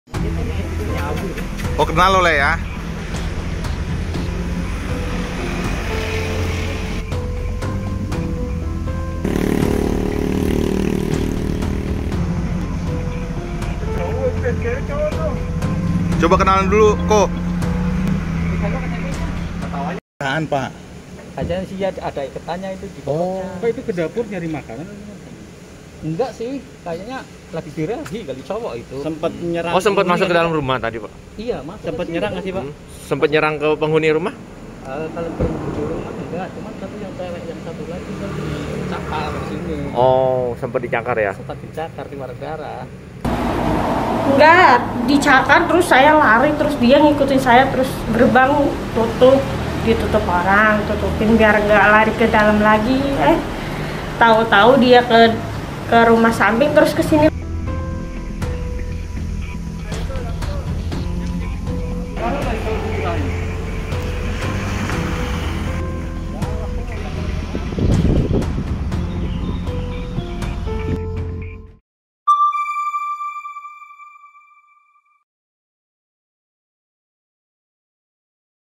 Monyet Berkeliaran di Permukiman Mewah Ancol, 1 Orang Mengalami Luka-Luka
Seekor monyet berkeliaran di permukiman mewah di Jalan Pasir Putih 3, Ancol, Pademangan, Jakarta Utara pada Senin (17/1/2022) siang. Monyet dewasa ini juga memasuki salah satu rumah warga, bahkan telah melukai salah satu orang penghuni rumah.

Android: inews.id/android
iPhone: inews.id/ios